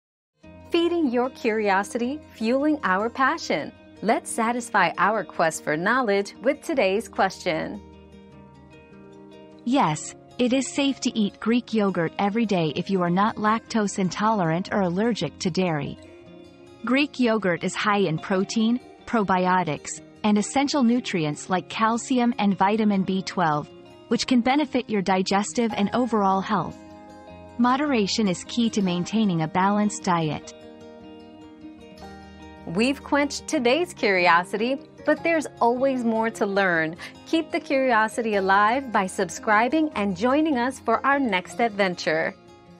Is it safe to eat Greek yogurt every day?
The Benefits of Eating Greek Yogurt Daily 👉 Greek Yogurt Benefits 👉 Discover the nutritional benefits of consuming Greek yogurt daily, including high protein, probiotics, and essential nutrients for digestive and overall health. Find out why moderation is key for a balanced diet.

Laura S. Harris (2024, June 14.) Is it safe to eat Greek yogurt every day?
    🌐 AskAbout.video